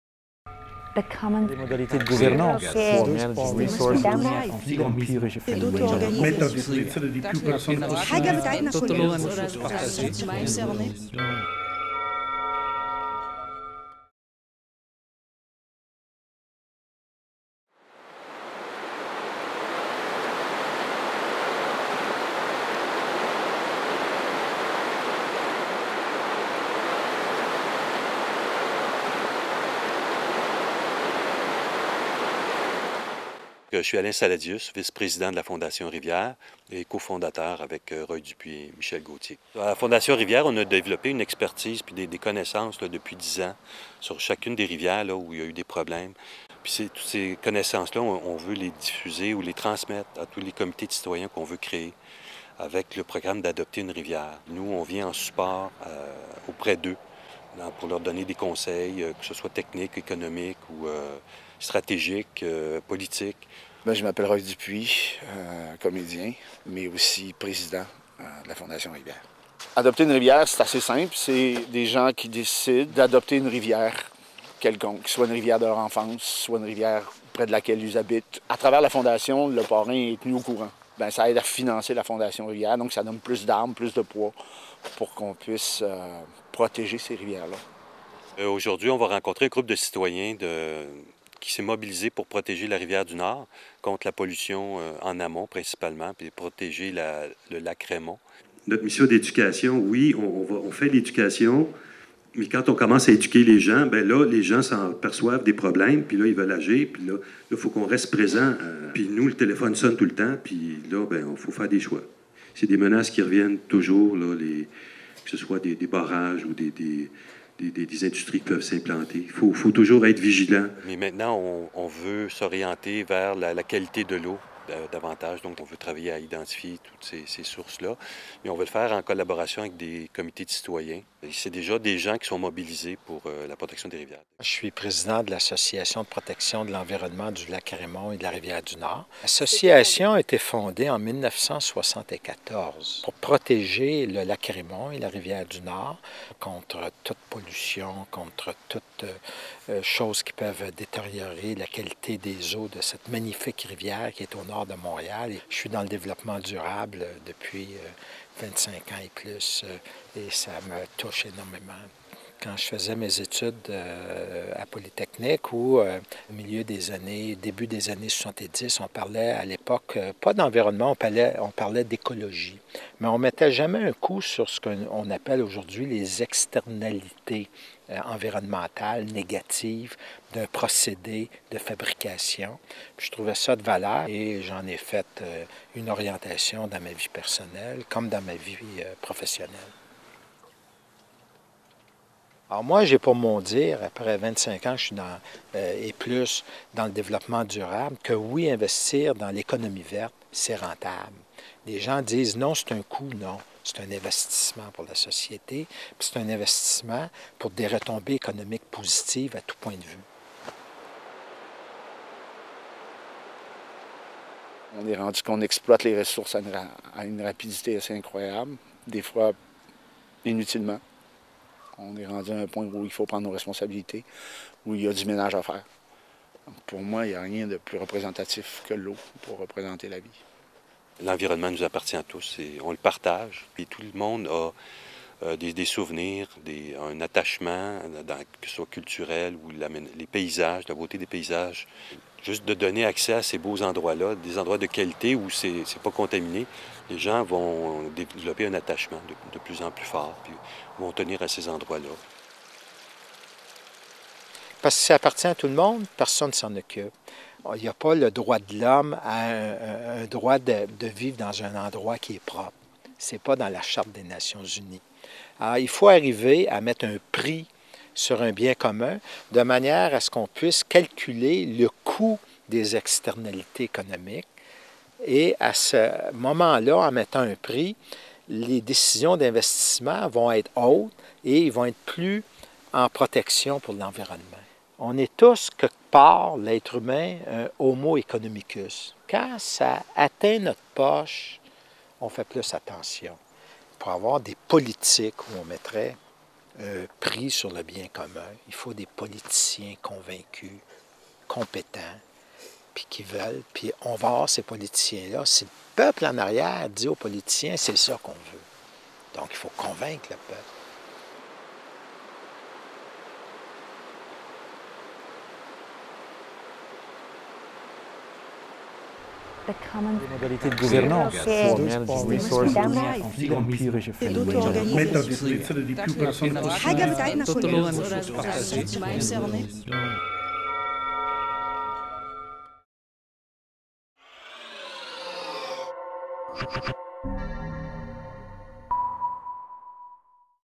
Ô Saint-Laurent: portrait isolé de la Fondation Rivières
Contexte:
Le fleuve St Laurent, icône de l'histoire québécoise, est actuellement difficile d'accès pour les Montréalais-es. Au cours des vingt dernières années, les processus d'urbanisation et d'industrialisation de la métropole ont éloigné la population de cette source d'eau. Cette ville insulaire a perdu le sens d'appartenance au fleuve. Dans le contexte actuel, nous nous posons la question suivante... Comment pouvons-nous nous ré-approprier le fleuve St Laurent comme bien commun ?

Une production de Communautique, dans le cadre du projet Remix biens communs.
Réalisation et montage : Stéphanie L-Bérubé
Images et entrevues : Stéphanie L-Bérubé et Émiliano Bazan